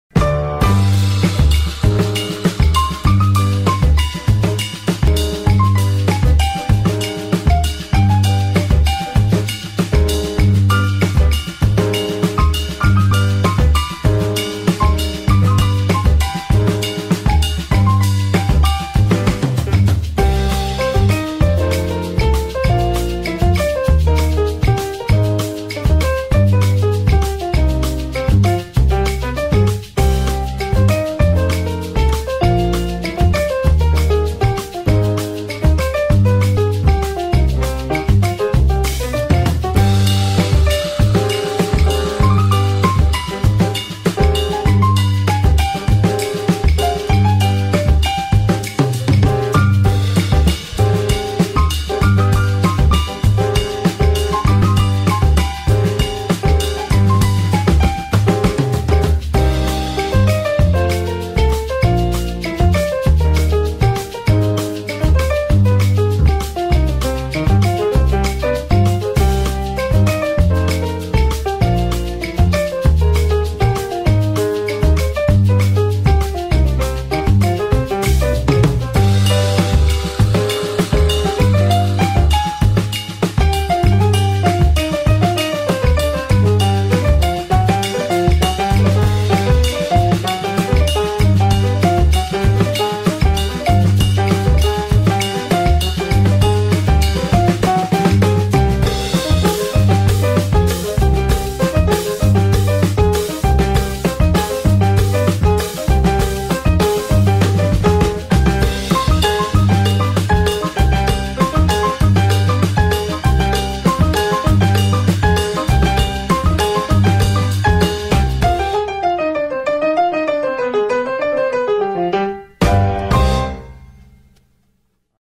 Havana Nights
Illustrative loose narrative of Cuban nightlife. Created as a scenic video look for a corporate party with a  "Havana Nights" theme.